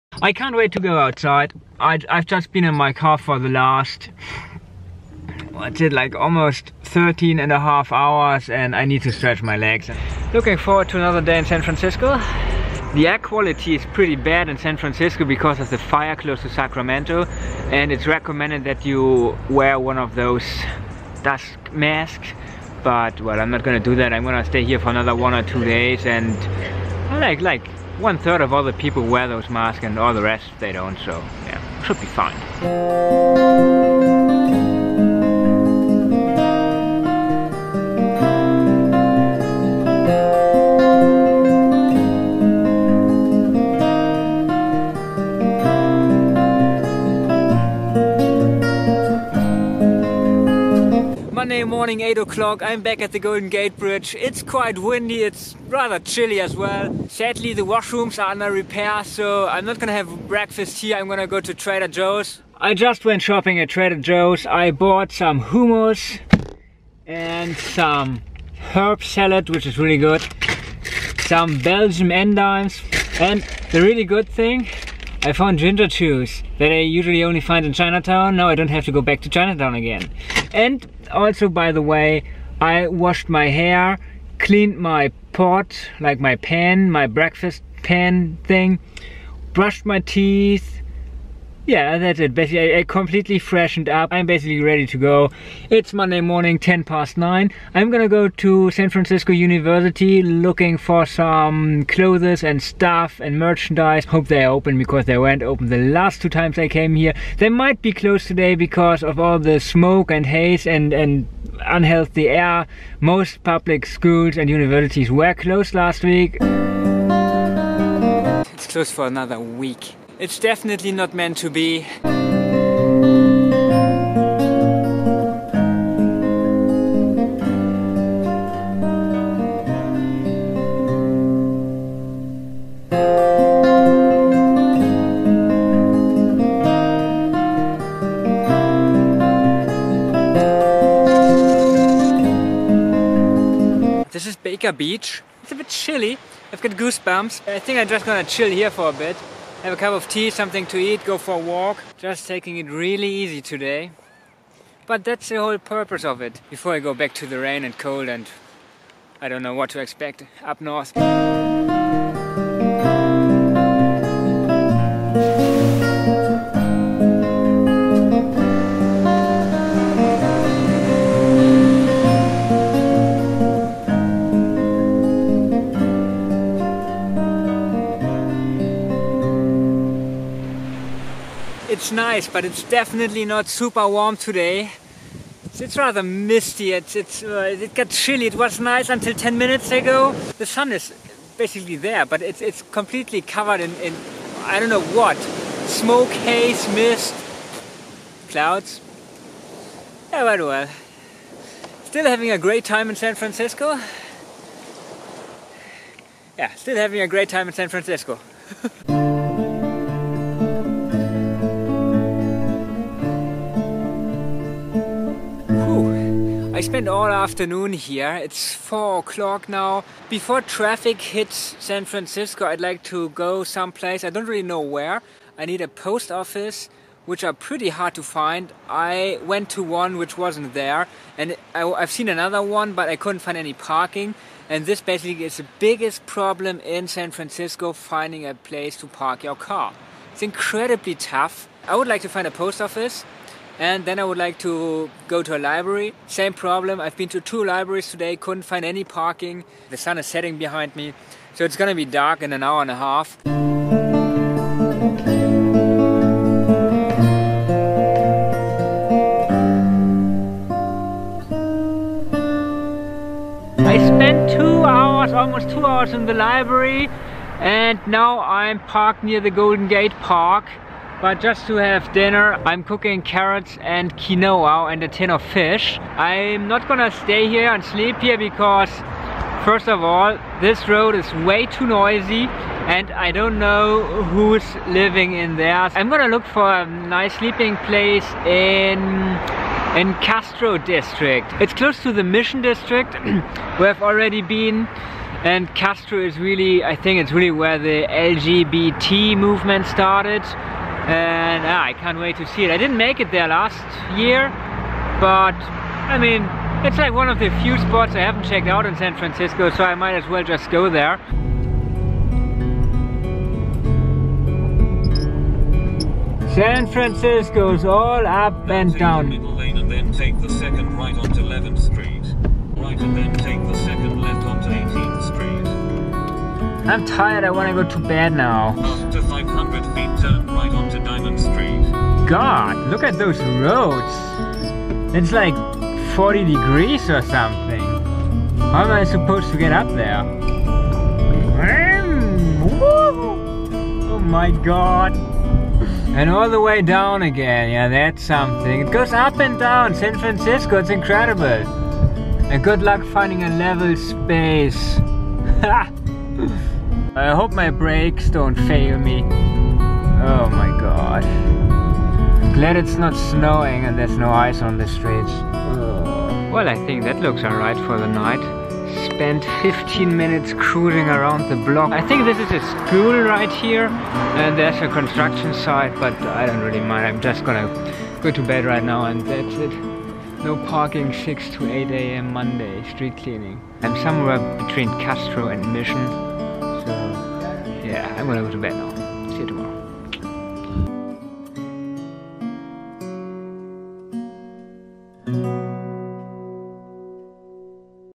HANNES GOES BANANAS ABROAD – FOG CITY
This video is about another guy traveling across the United States while living in his van and talking to his cameras. The cameras used in this video are a Canon G7X Mark II, a Panasonic DMC-GX80, a YI 4K+ Action Camera and a DJI Spark.